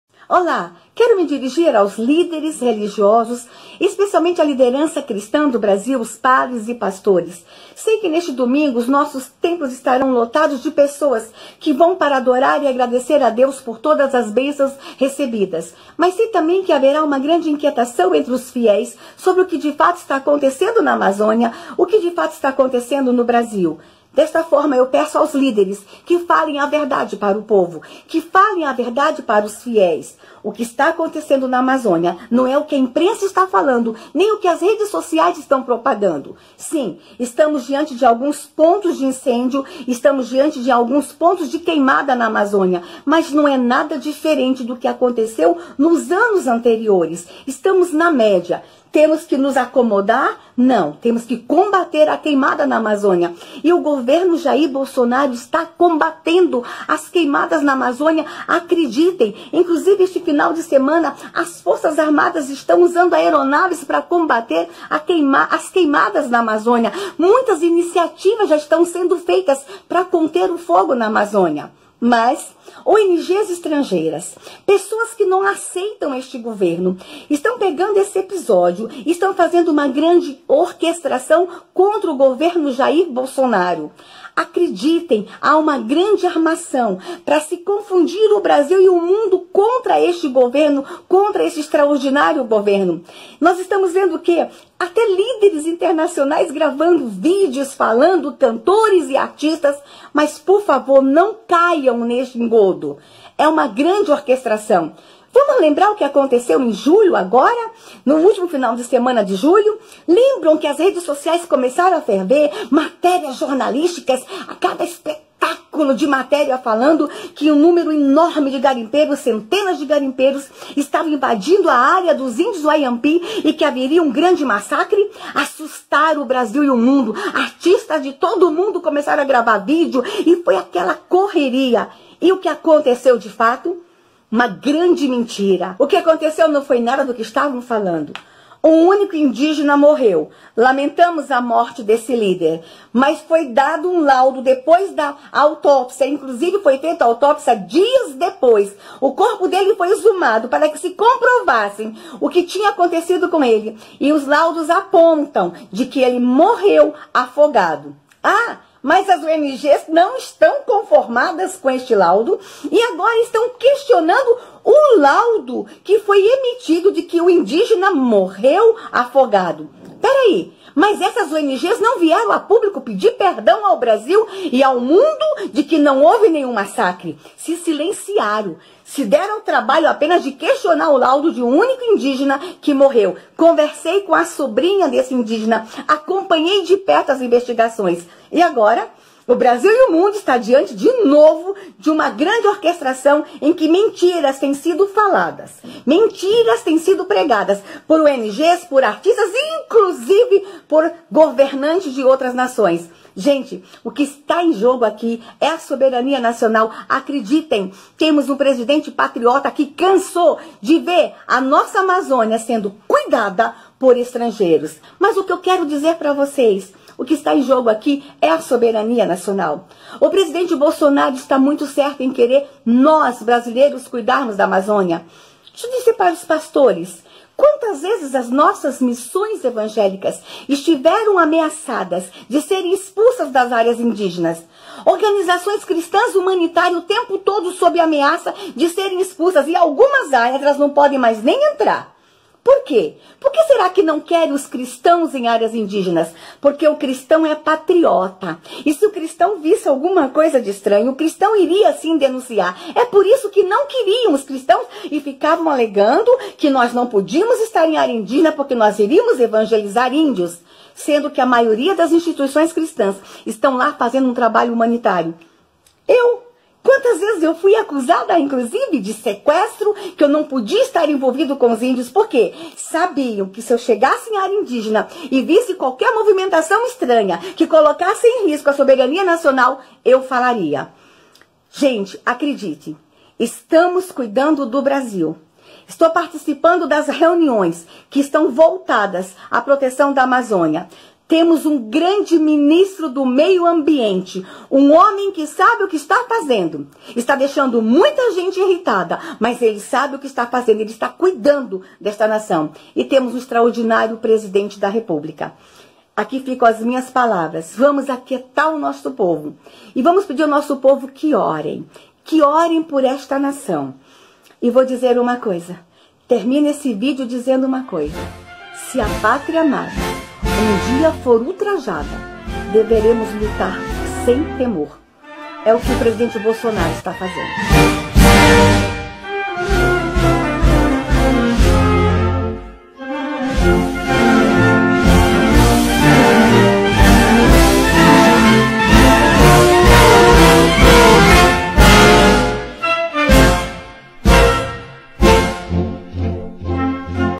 Ministra DAMARES fala ao povo cristão brasileiro
OLÁ POVO CRISTÃO...
Não deixem de ouvir as palavras sábias da nossa ministra. É fato que as notícias vindo da Amazônia estão causando muita confusão e precisamos nos posicionar...